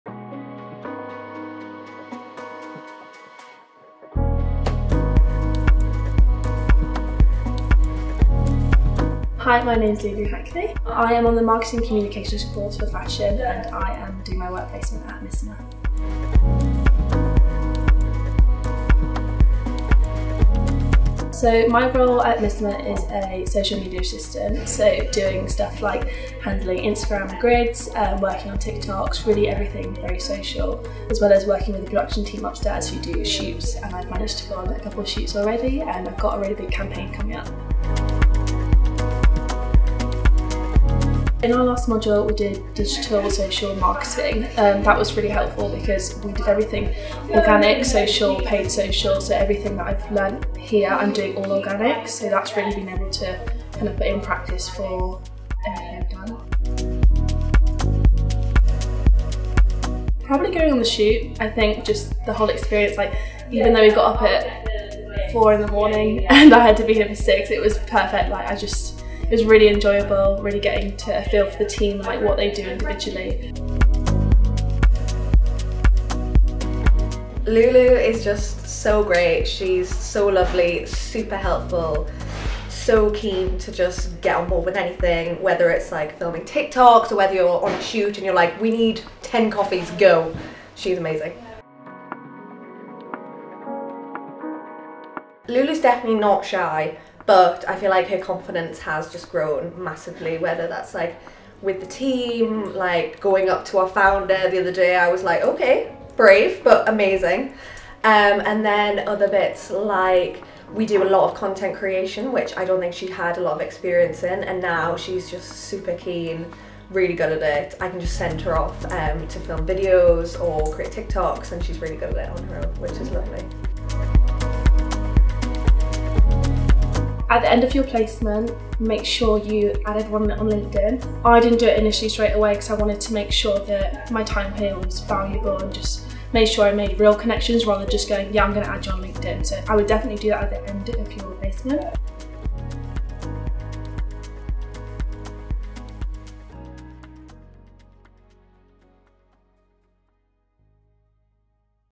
WHAT WILL YOU GET UP TO ON YOUR WORK PLACEMENT!
We offer 3 week full time work placements for our students!
We visited Lulu at Missoma to see what she got up to during her work placement.